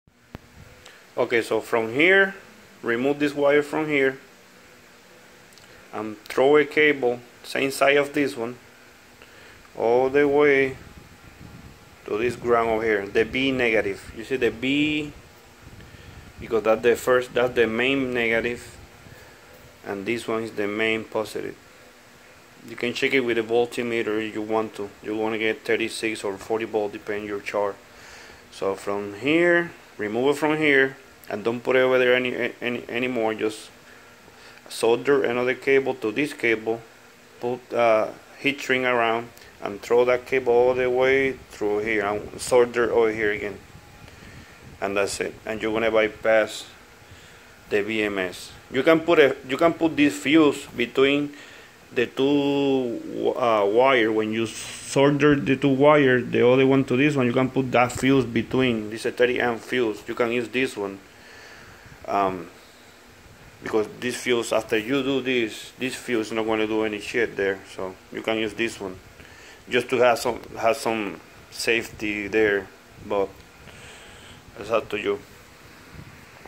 bird 590 bms bypass
so guys let me put something very clear for those people that dont know anything about bms, by doing this method you only will bypass the current draw side of the  bms not the charging side of the bms. never never bypass the charging side of a bms its very very dangerous consequences can be catastrophic.